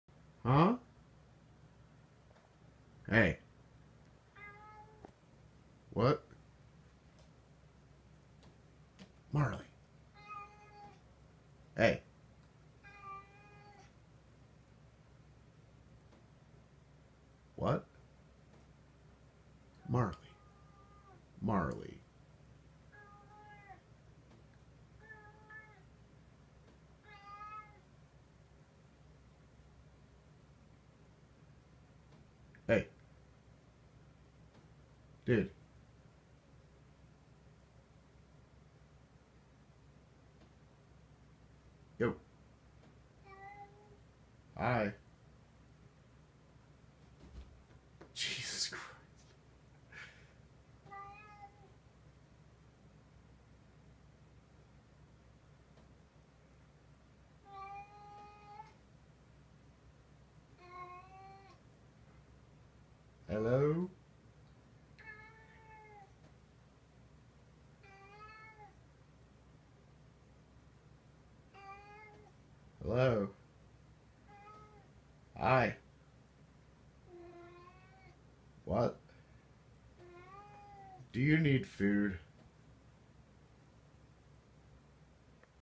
Sunday Morning Conversation with a Cat
Marley is a loudmouthed klutz.